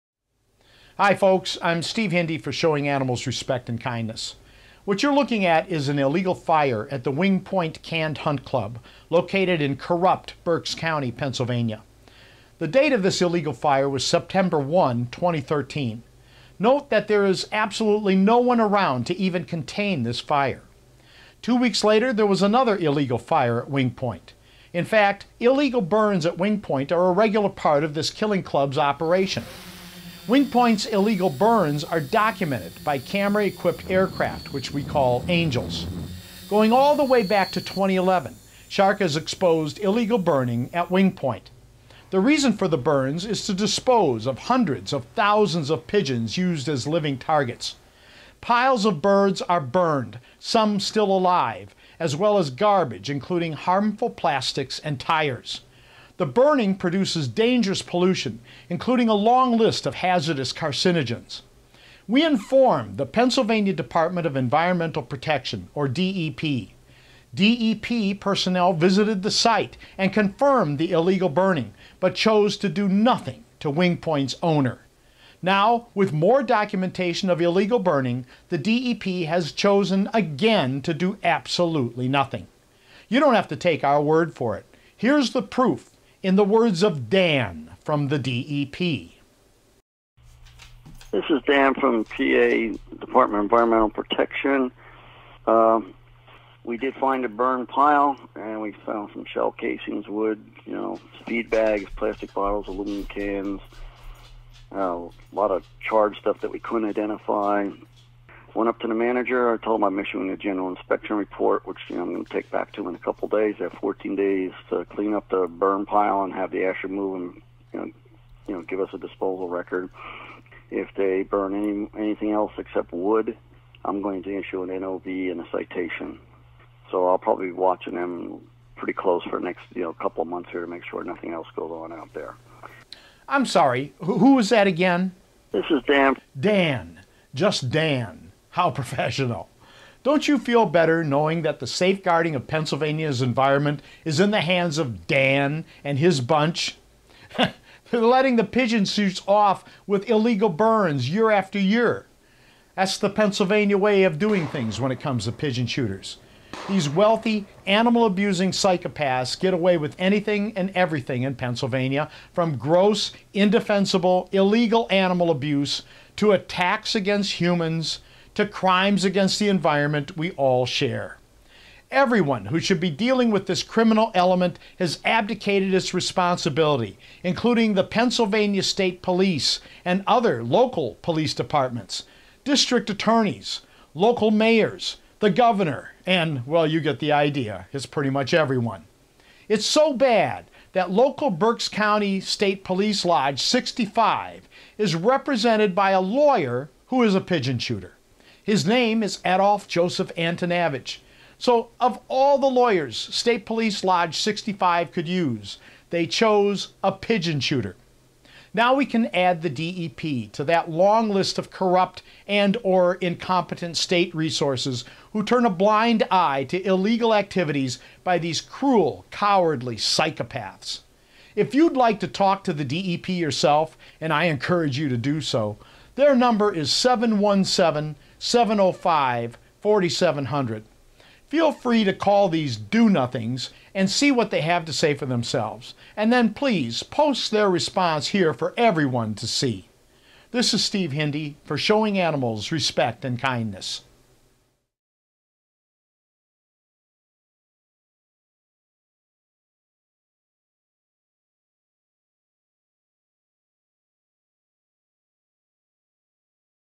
Drone Catches Pennsylvania Polluter
SHARK has repeatedly exposed the illegal burning by a club involved in live pigeon shoots. With clear, irrefutable video evidence, why does the DEP refuse to do its job?